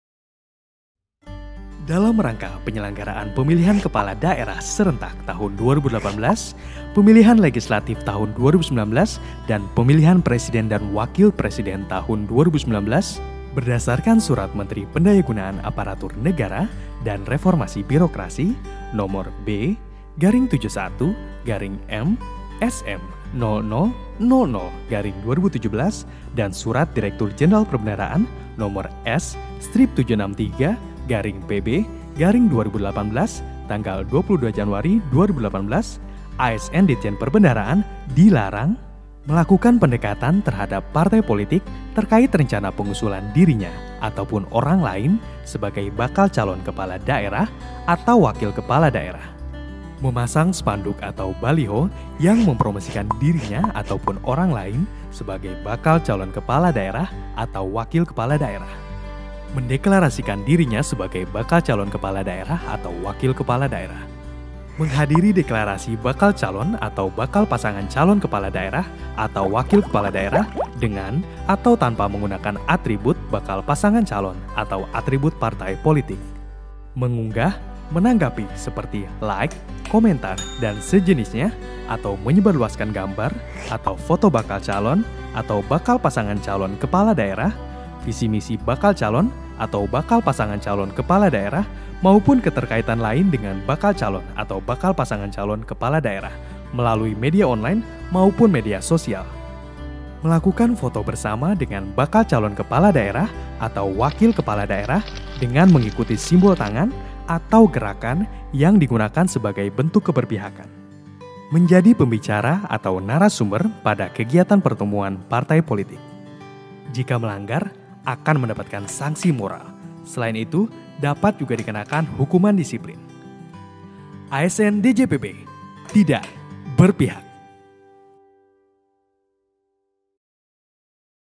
ASN DJPb Tidak Berpihak - Netralitas Menjelang Pemilu 2019
Sebagai Aparatur Sipil Negara, kita dituntut untuk bersikap netral dalam  pemilu sesuai yang tertulis dalam surat Menteri PANRB Nomor B/71:/M.SM.00.00/2017

Simak video berikut supaya kita mengetahui apa saja yang dilarang dalam menyikapi Pemilu 2019